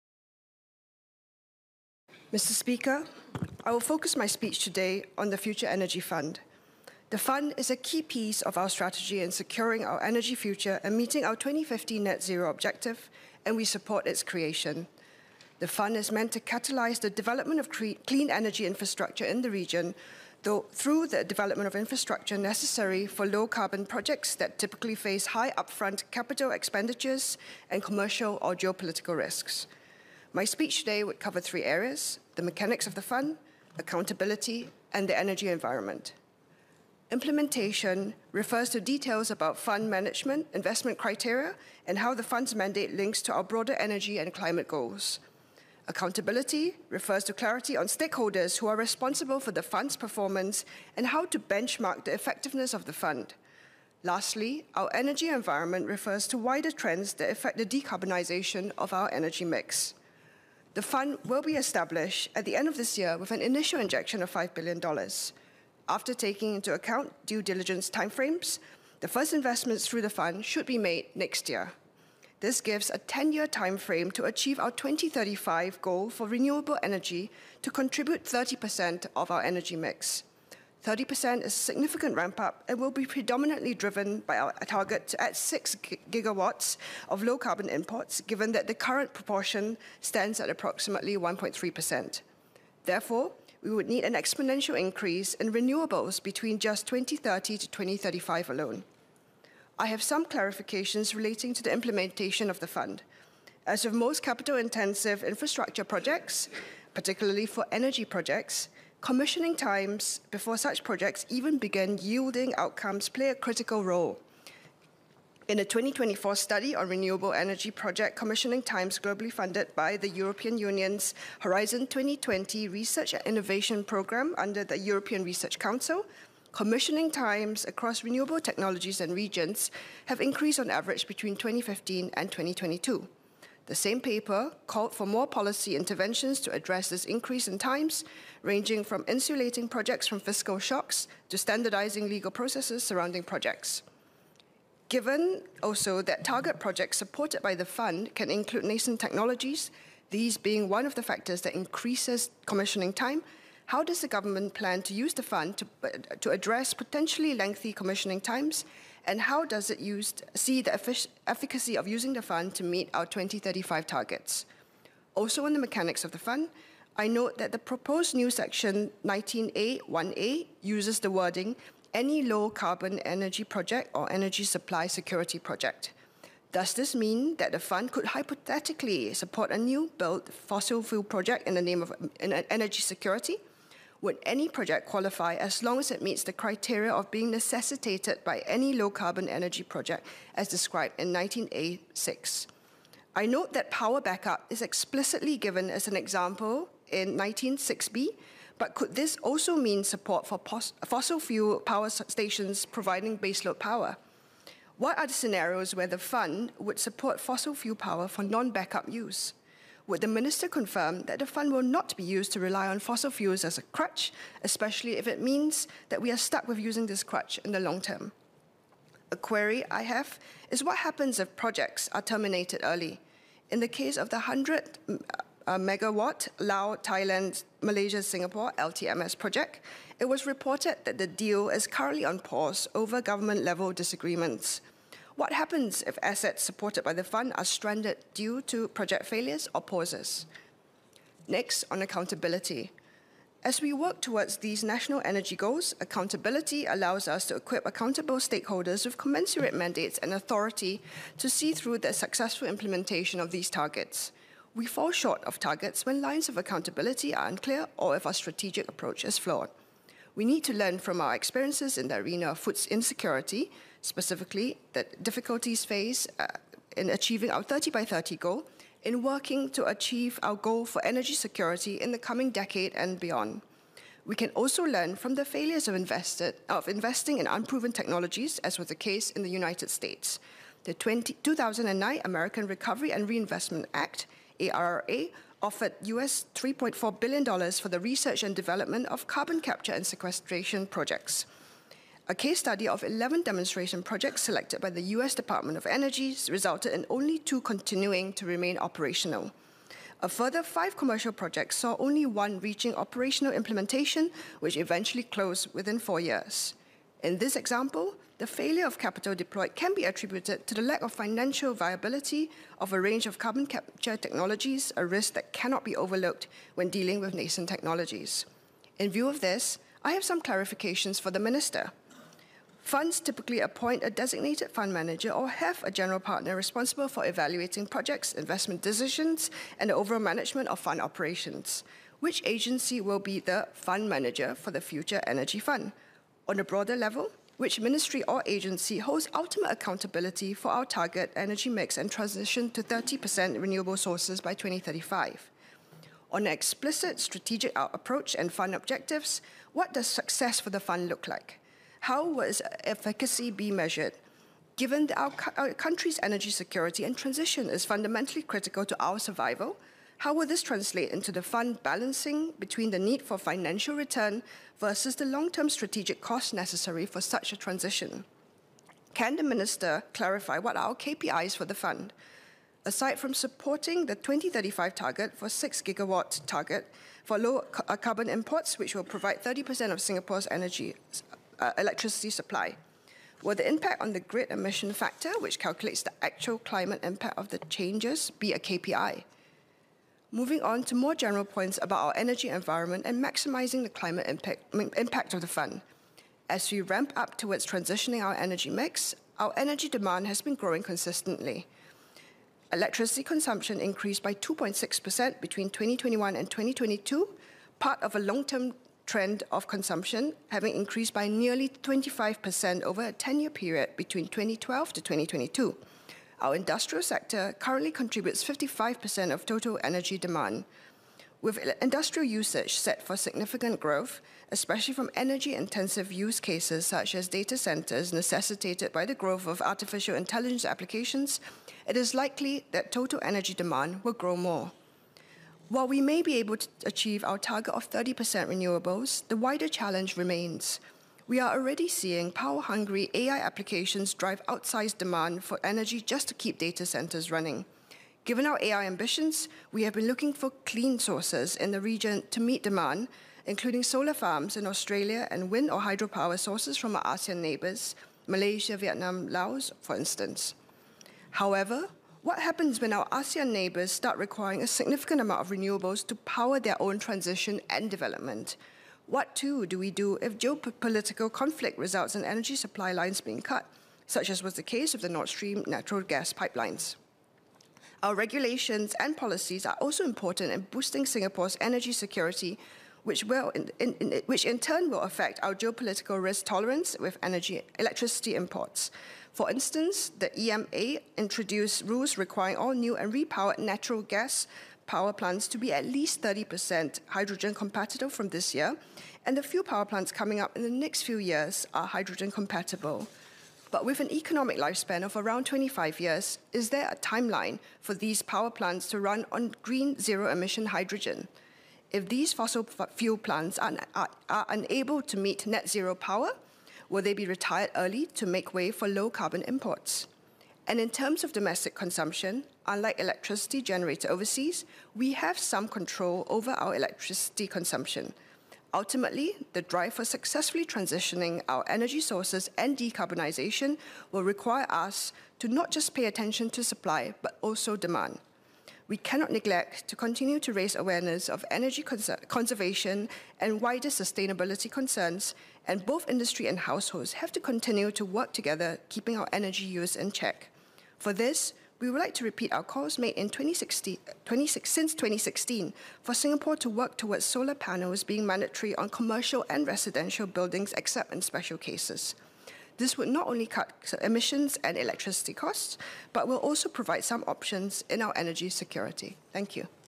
MP He Ting Ru - Parliamentary Speech on Energy Transition Measures and Other Amendments Bill
In her speech on the "Energy Transition Measures and Other Amendments Bill" (9th September 2024), MP He Ting Ru emphasised the Future Energy Fund's role in securing Singapore’s energy future and reaching its 2050 net-zero target. While supporting the S$5 billion investment in clean energy, she raised concerns about fossil fuel reliance, project risks, and the need for accountability and strategic planning.